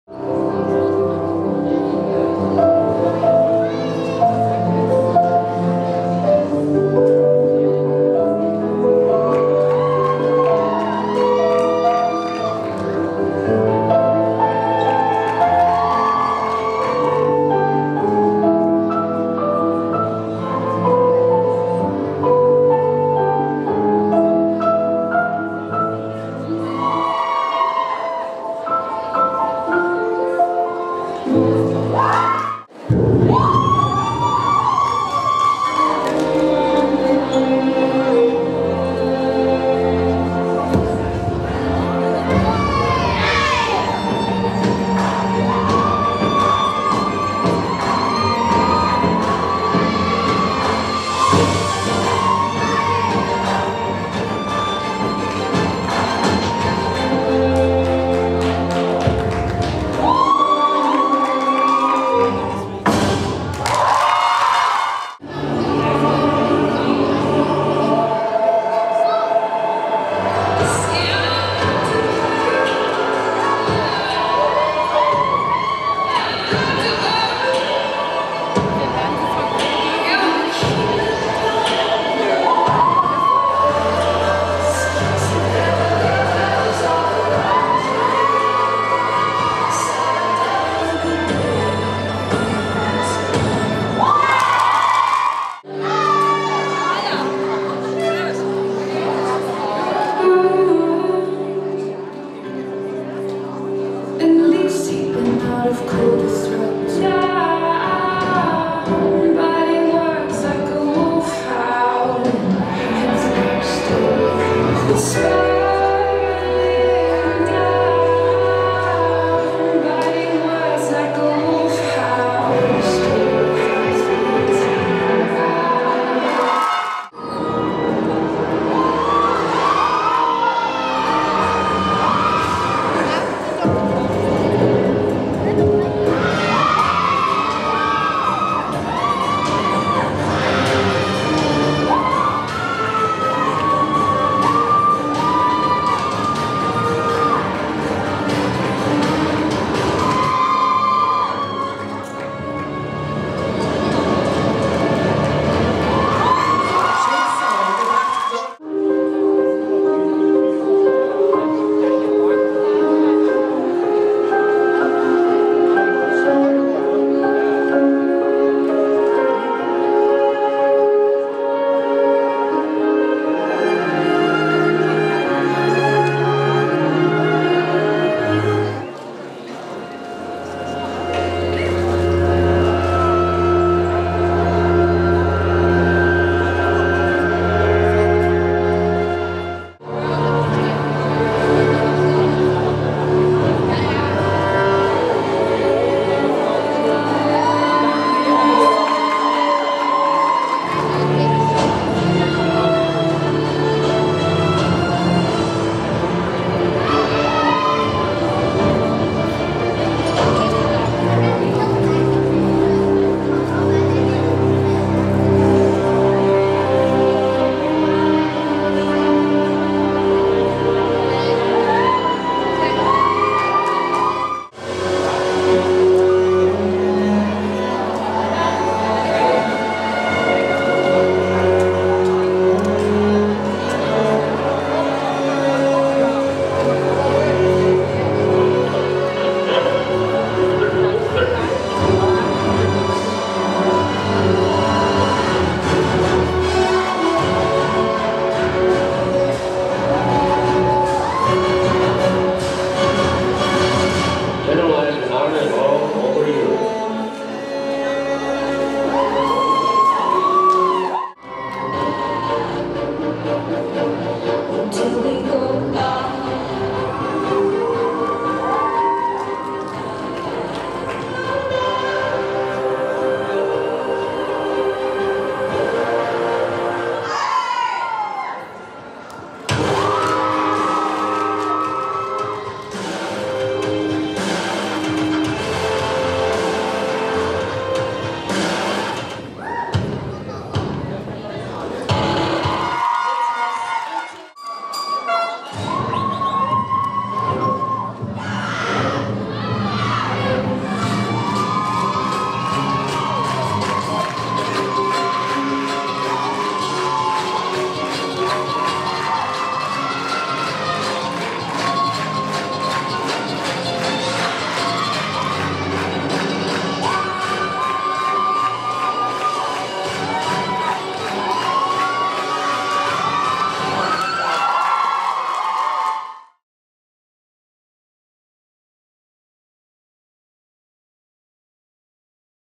DF Silverstar op Belgisch kampioenschap
Danceformation Silverstar de winnaar van de 12 gouden medaille op BK (Belgisch kampioenschap 2019))